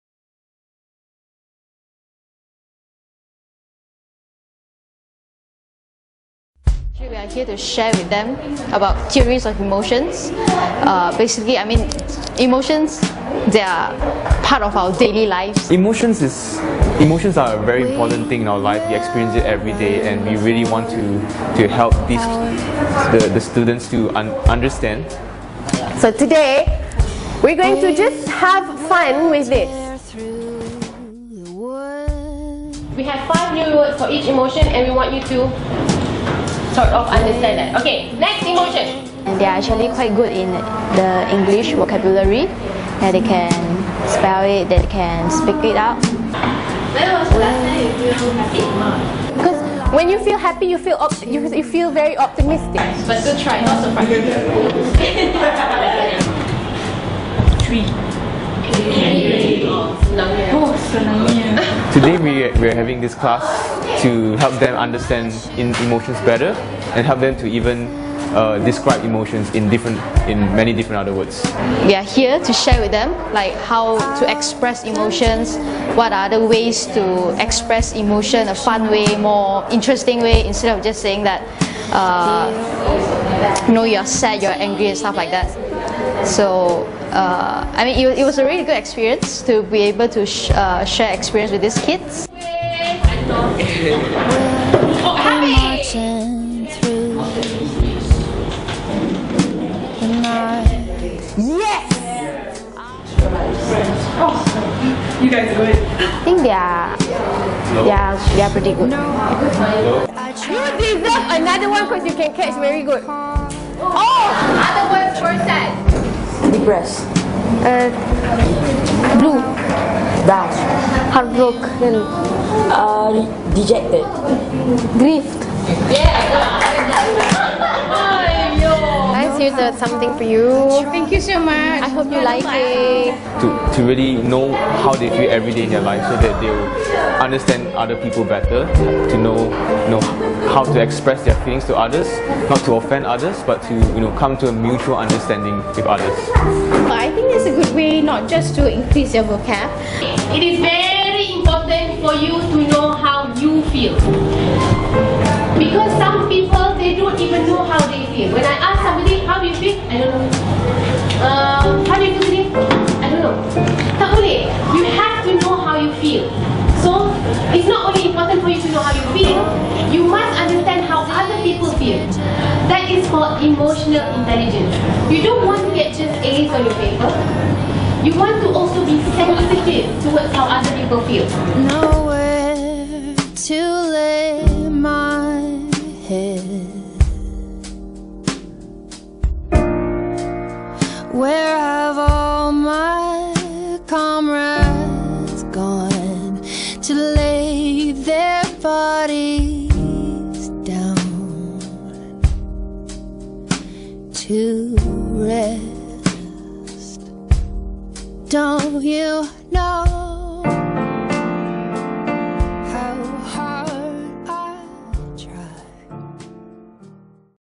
Wardina and her team on Emotional Intelligence and Vocabulary
This is a group assignment lead by Wardina and her team from Help Institute, explaining to the students of the importance of EI and Vocabulary.